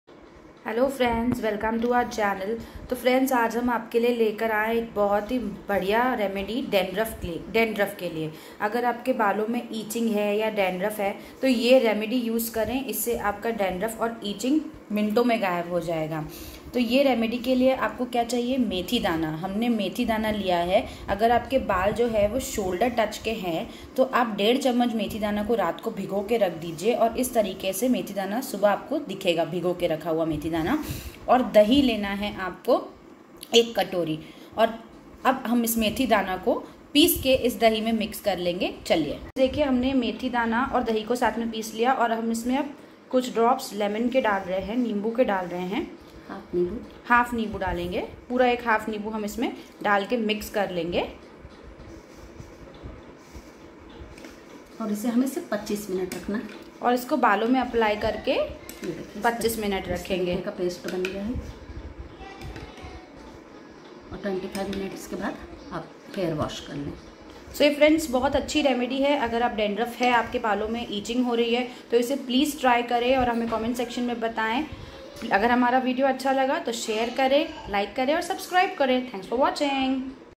इस तरह से पाये dandruff से छुटकारा
in this video we have shown a simple remedy to clear dandruff